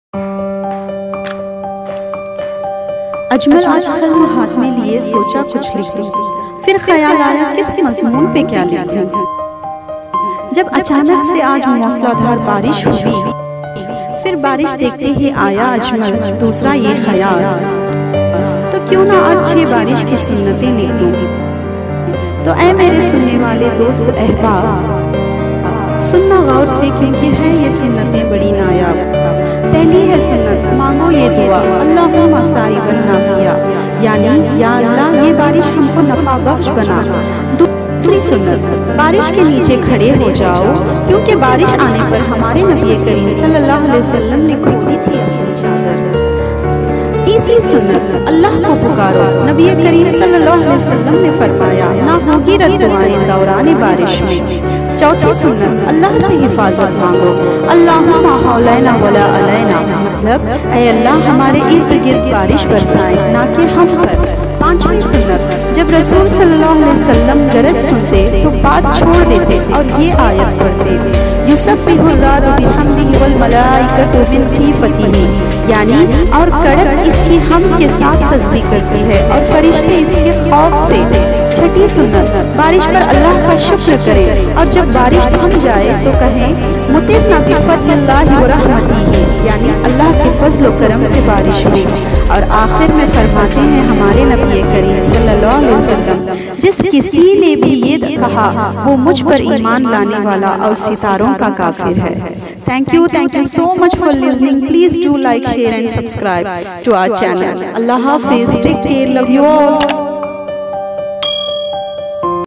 Baarish Ki Sunnatein | Sunnahs During Rain 🌧 | Shimoga
Assalam Valaikum All,

Baarish Ki Sunnatein  | Sunnahs During Rain 🌧

In shimoga heavy rain is happening and it's very unfair to the people who don't have proper shelters but we can't say it unfair because it Allah swt plan.. for that our prophet Mohammed ﷺ taught us to follow these sunnahs.. we hope this will be a helpful for us all..

Allah protect us from all the bad deeds and protect us by the wrath of allah ..

ameen sumameen :)